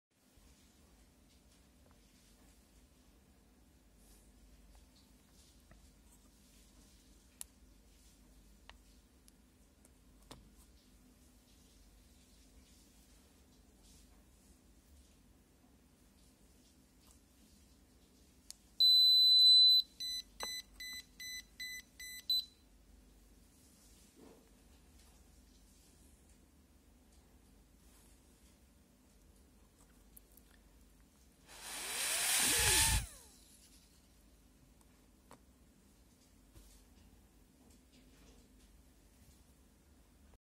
Connect to Wind Meter and/or BlueFly Variometer device
Wind Meter anemometer (air speed data) and BlueFly vario (barometer for precise altitude data) are two devices supported now by the aero system CdaCrr